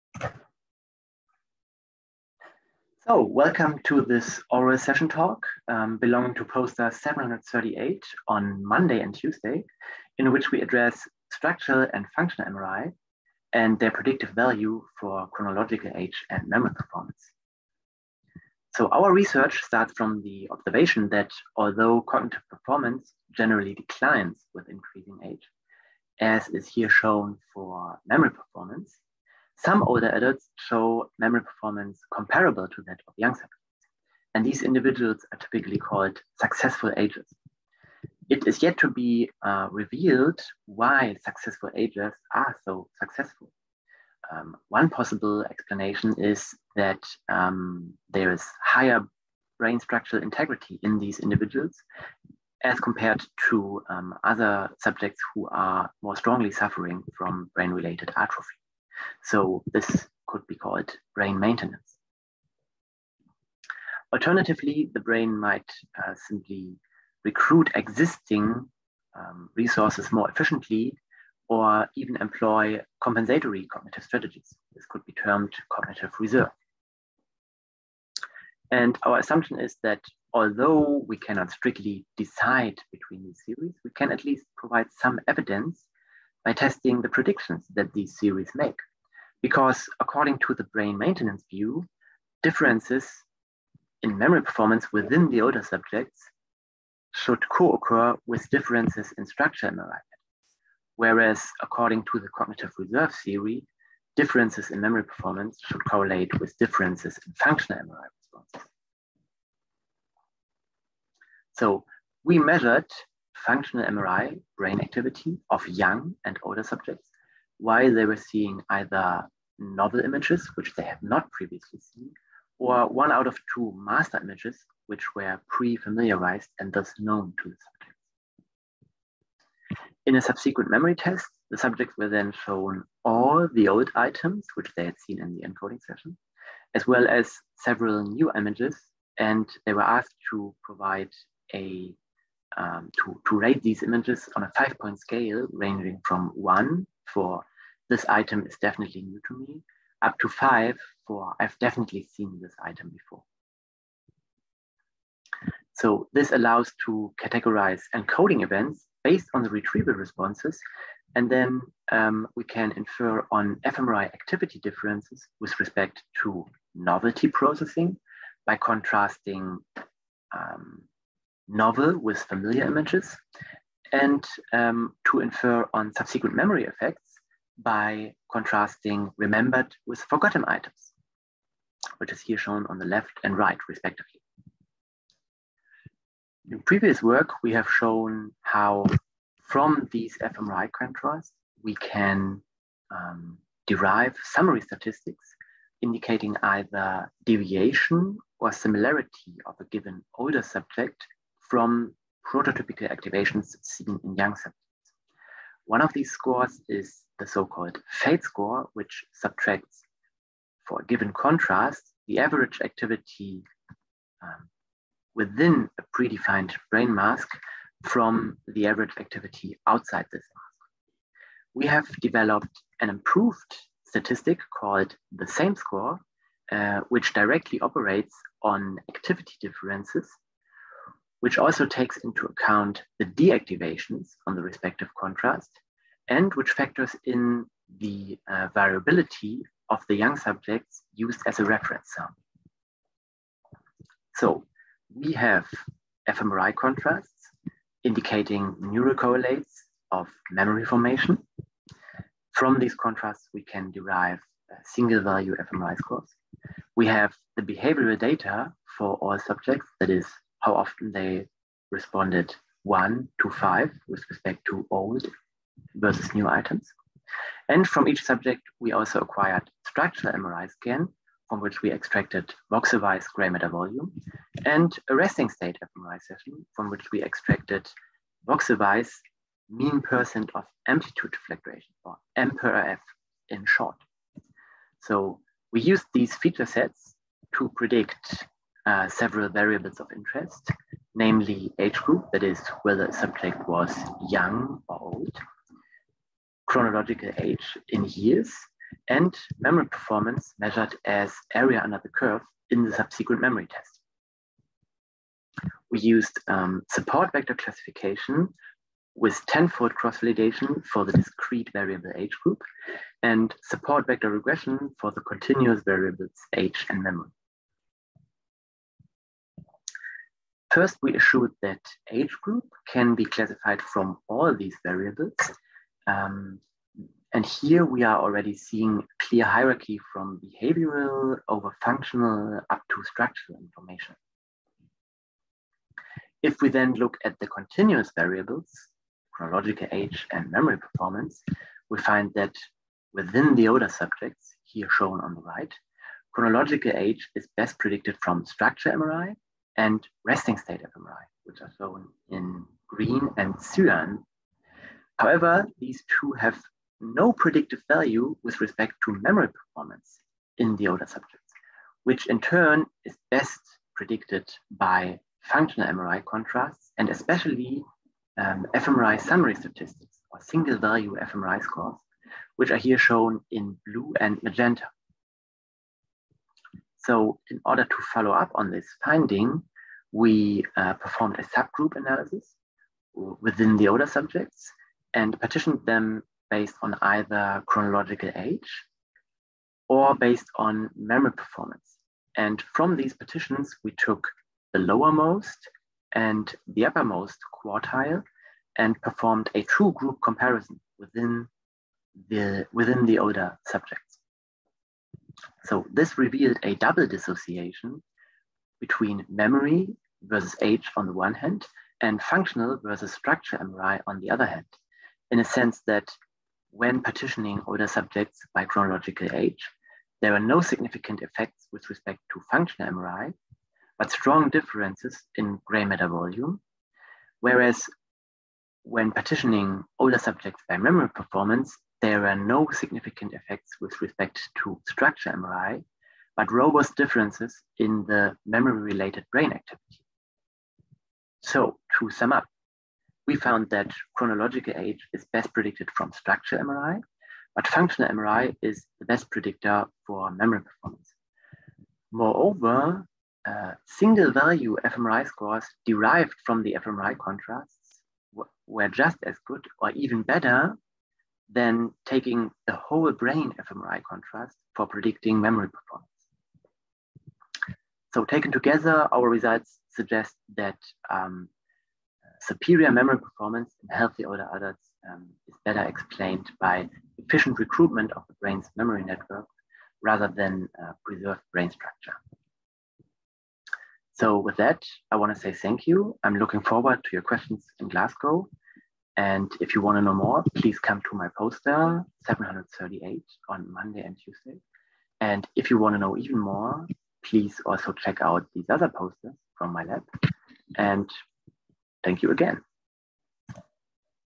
OHBM 2022 – Oral Session Talk: "Structural and functional MRI predict age and memory"
28th Annual Meeting of the Organization for Human Brain Mapping (OHBM)
Oral Session: "Learning and Memory: How Experience Changes the Brain"
Talk Title: "Structural and functional MRI data differentially predict chronological age and memory performance"